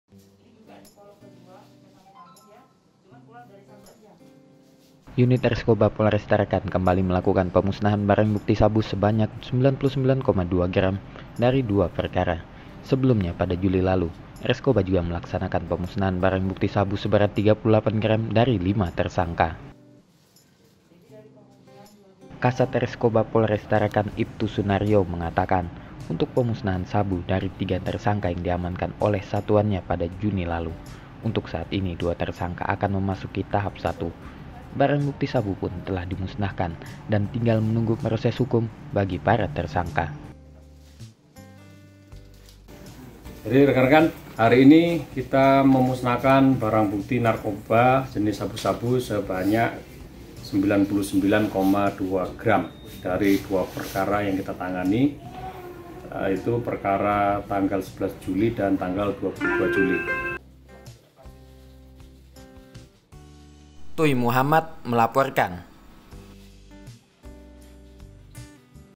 RESKOBA KEMBALI MUSNAHKAN BARANG BUKTI SABU
UNIT RESKOBA POLRES TARAKAN KEMBALI MELAKUKAN PEMUSNAHAN BARANG BUKTI SABU SEBANYAK 99,2 GRAM DARI DUA PERKARA, SEBELUMNYA PADA JULI LALU RESKOBA JUGA MELAKSANAKAN PEMUSNAHAN BARANG BUKTI SABU SEBERAT 38 GRAM DARI LIMA TERSANGKA.